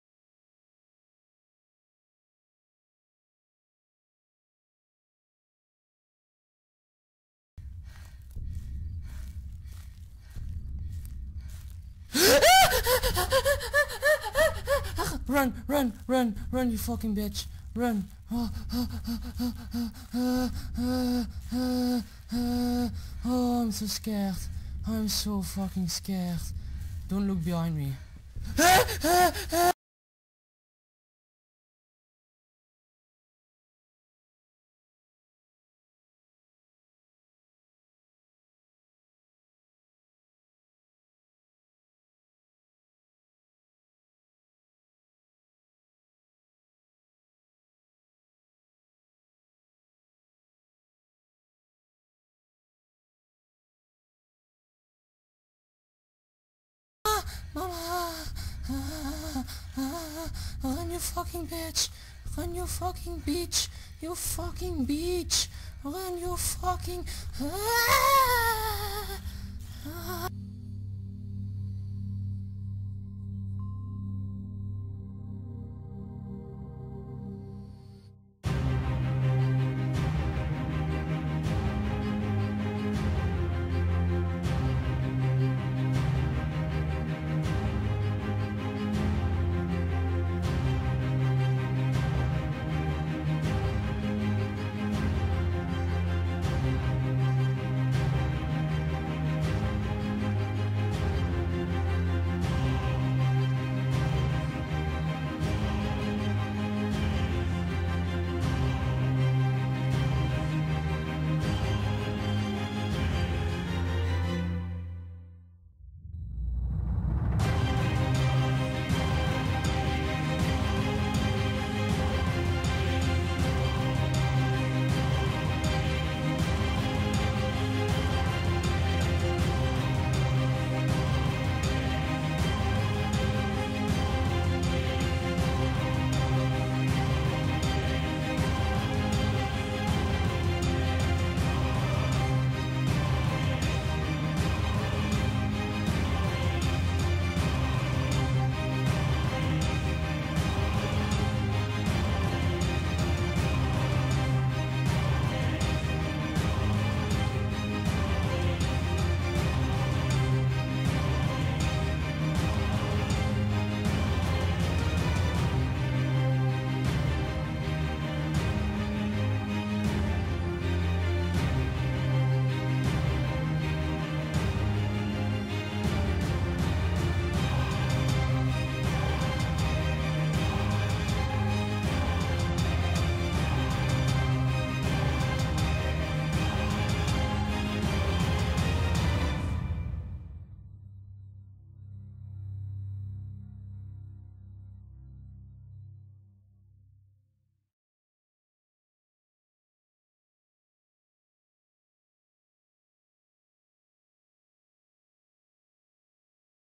You choose I play!
pick a number between 1 and 5 and choose witch horror game I will play.

goodluck!

Thank you very much and have a nice day!!